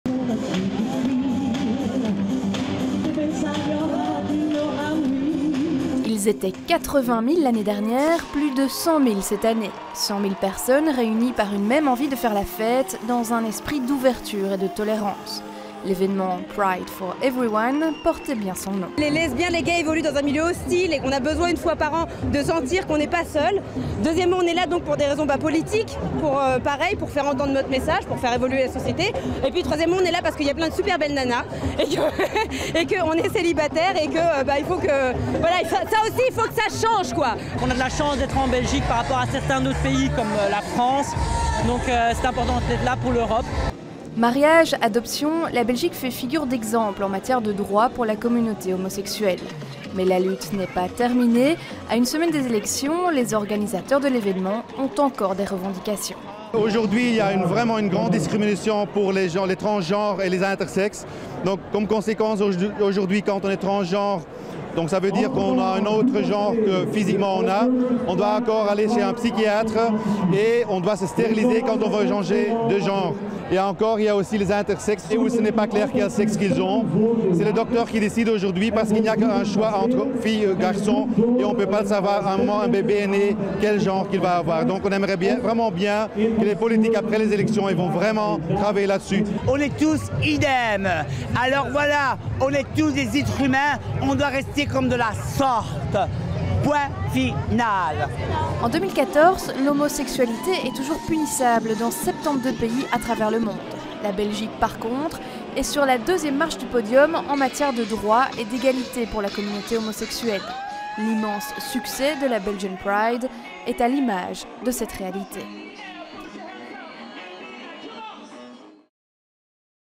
Belgian Pride à Bruxelles
Belgian Pride dans les rues du centre de Bruxelles.